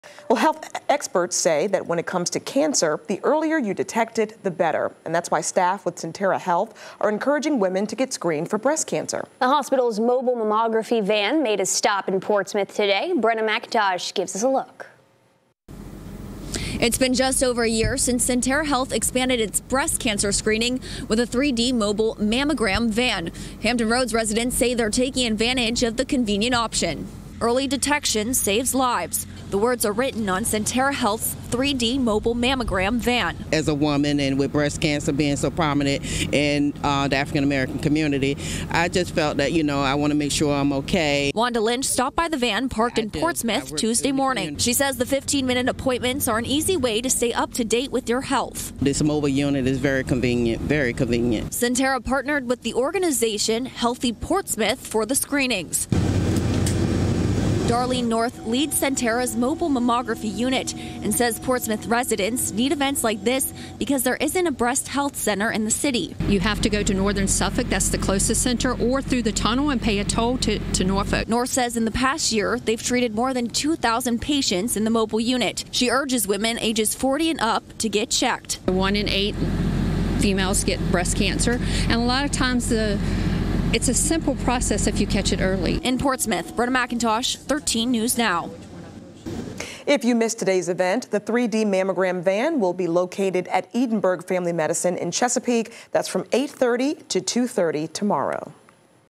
Sentara's mammography van sees more than 2K patients in first year
Hampton Roads residents say they’re taking advantage of the convenient option.